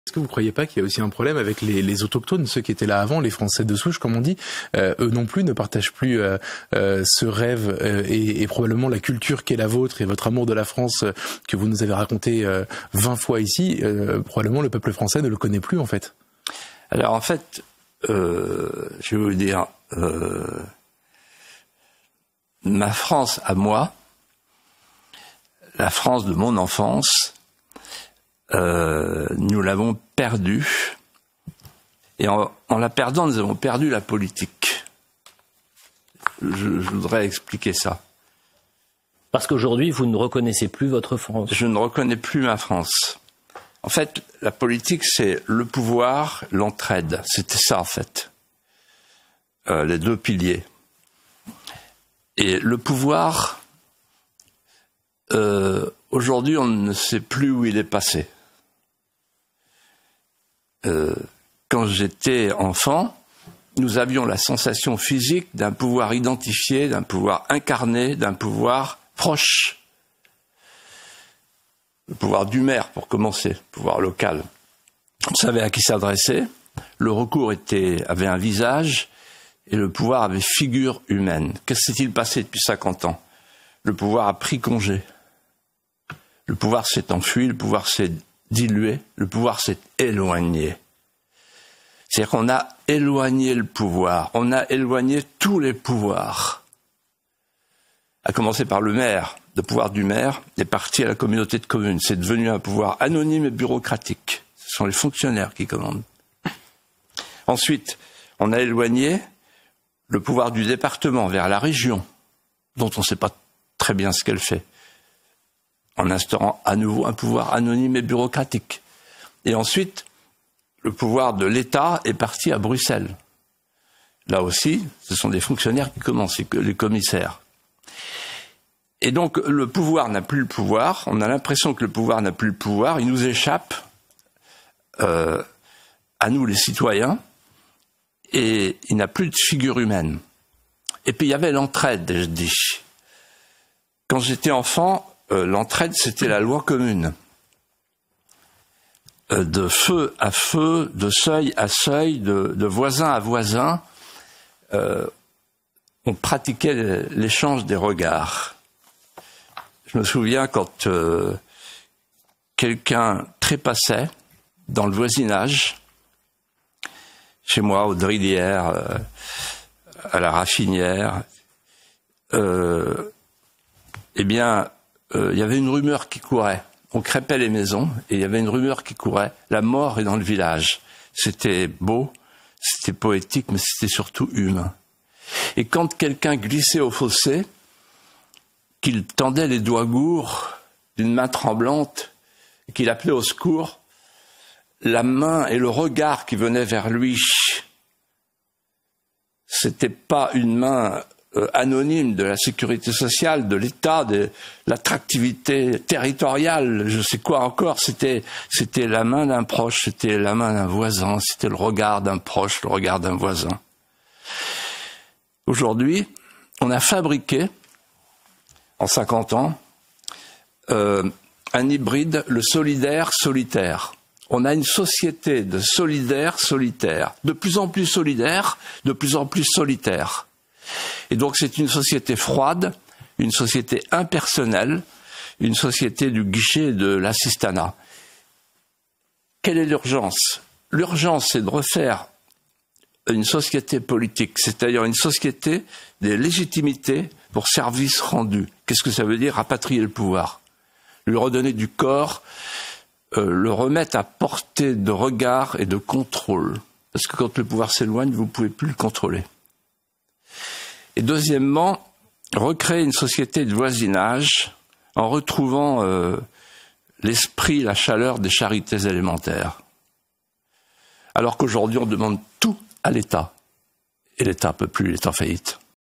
Philippe de Villiers : "Je ne reconnais plus ma France"
🎙️Philippe de Villiers : "Ma France à moi, la France de mon enfance, nous l'avons perdue.
On ne sait plus où est passé le pouvoir : il n'est plus identifié, incarné, proche.
En 50 ans, le pouvoir a pris congé : il s'est éloigné à Bruxelles.
On a fabriqué une société 'solidaire-solitaire', froide, impersonnelle."

📺Emission du 19 avril 2024, CNews.